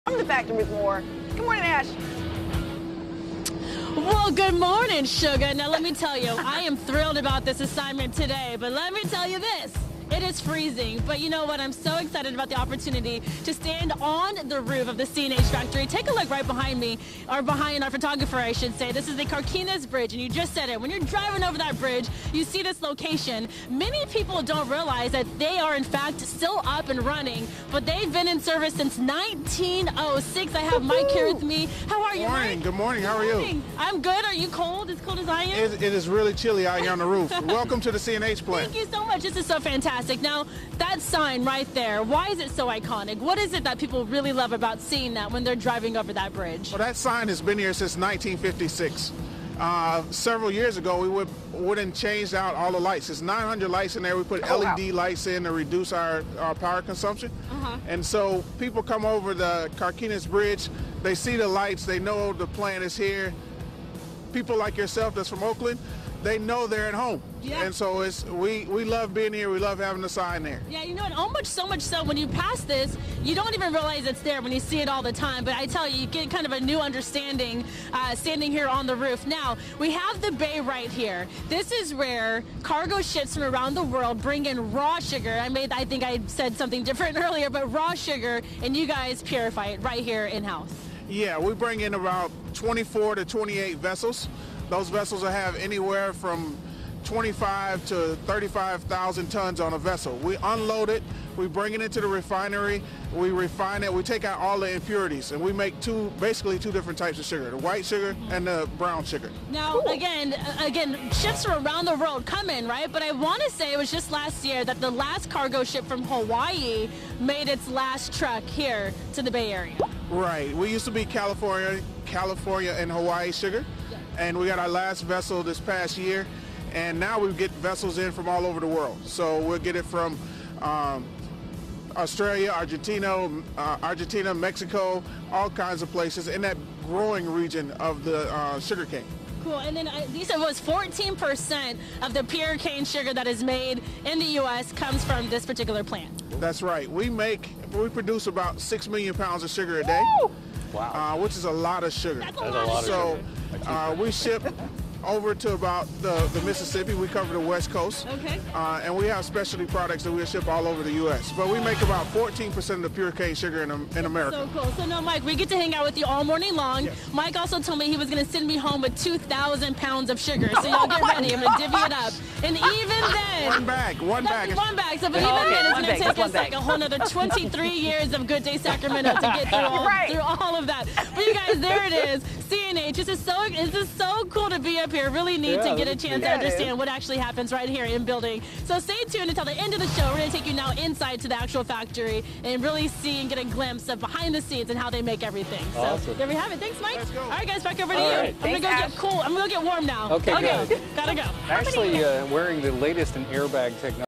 Inside The C&H Factory
Ashley Williams is at the C&H Sugar Factory to learn about its history and see how sugar is made!